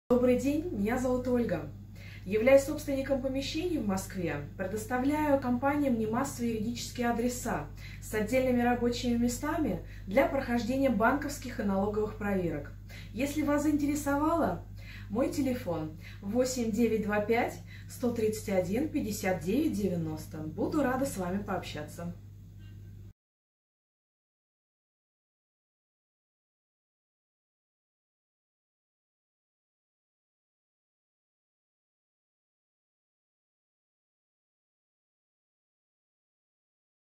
НЕМАССОВЫЕ ЮРИДИЧЕСКИЕ АДРЕСА В МОСКВЕ
Ищите юридический адрес для своего бизнеса или компании?

Предлагаем НЕМАССОВЫЕ ЮРИДИЧЕСКИЕ АДРЕСА В МОСКВЕ от собственника помещений

НАШИ ПРЕИМУЩЕСТВА:
• Вместе с юридическим адресом вы получаете: чистый, светлый и недорогой меблированный офис
• Юридические адреса c почтовым обслуживанием
• Юридические адреса с возможностью банковских проверок
• Юридические адреса с возможностью выездных налоговых проверок
• 100% гарантия прохождения в банке при открытии счёта.